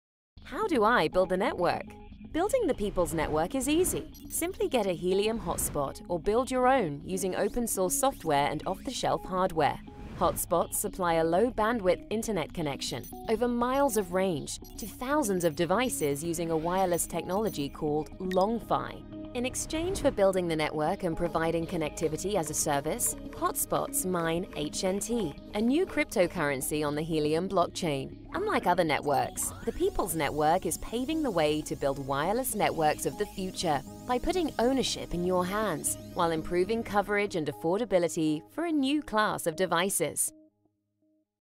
How do I mine HNT - iHub Global
A new way to mine crypto
By deploying a simple device in your home or office, you can provide your city with miles of low-power network coverage for billions of devices and earn a new cryptocurrency, HNT.